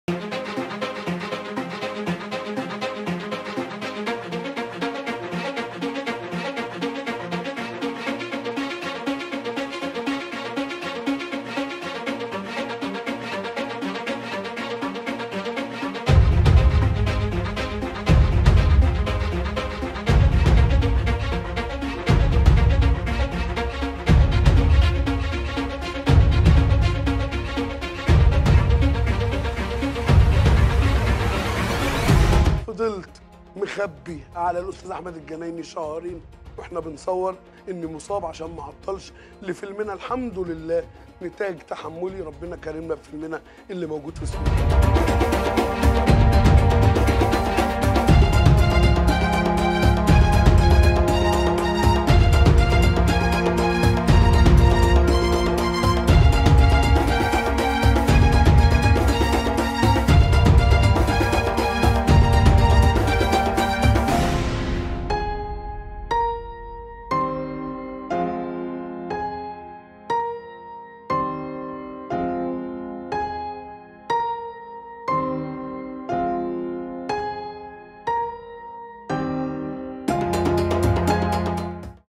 بتر قدم الفنان شريف دسوقي الشهير بسبعبع
القصة الكاملة لبتر قدم الفنان شريف دسوقي
أعلنت بدرية طلبة عن إجراء الفنان شريف دسوقي لعملية جراحية. أسفرت العملية عن بتر قدمه.أصيب شريف خلال  كواليس مشهد من فيلم «وقفة رجالة»

جريدة الوطن
جريدة يومية مستقلة